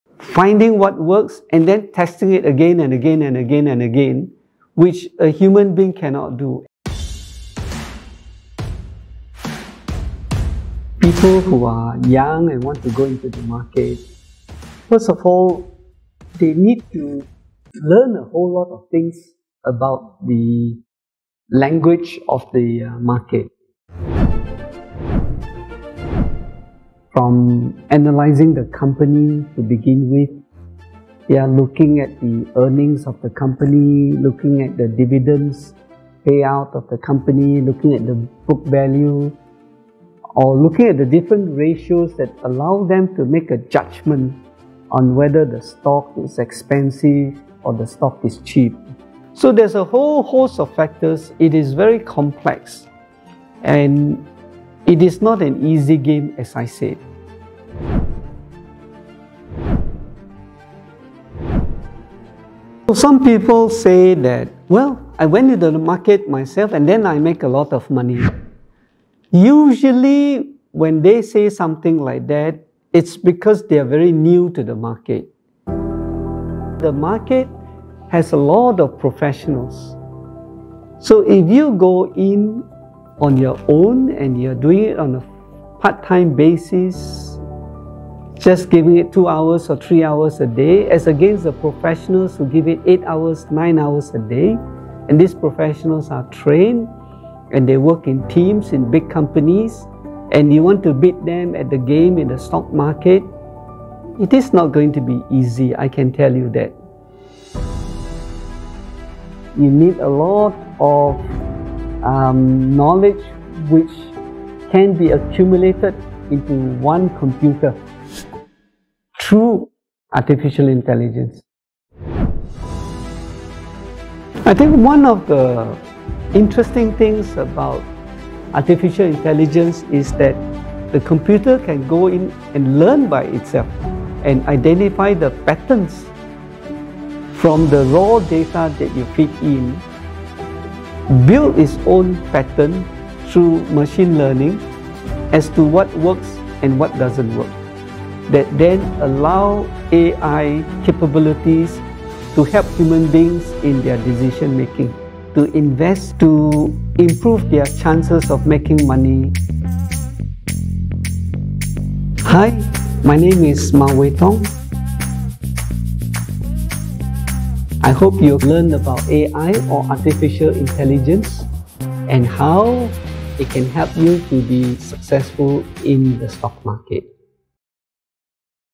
Artificial Intelligence for Stock Trading
Learn how Wei Tong( ex-CEO of Singapore's top investment firm), used  technology to grow his personal wealth 🧩

Artificial Intelligence (A.I.) has improved our lives in many areas. We have heard how computer programs can beat the world's best chess and go players. It will be a norm to ride in a driverless car soon. And the exciting news for all traders and investors is that they can start using A.I. for stock investing.

AlgoMerchant team, the first Singapore data scientist, has 𝐬𝐮𝐜𝐜𝐞𝐬𝐬𝐟𝐮𝐥𝐥𝐲 𝐢𝐝𝐞𝐧𝐭𝐢𝐟𝐢𝐞𝐝 𝐦𝐚𝐫𝐤𝐞𝐭 𝐢𝐧𝐞𝐟𝐟𝐢𝐜𝐢𝐞𝐧𝐜𝐢𝐞𝐬 to consistently generate alpha return performance of more than 15% per annum for retail investors. And it is known for its success in avoiding the recent major crash such as:

✅ US-China Trade war(2018)
✅Covid Pandemic(2020)
✅ Russia-Ukraine War (2022)

Many top-tier hedge funds have adopted a similar approach in secrecy to beat 98% of investors out there. This webinar will demystify how A.I. can learn, analyze, trade, and adapt in a dynamic market. It is 100% science.

and REGISTER our upcoming webinar today to get the latest insight on leveraging A.I. technology to gain an edge in the stock market.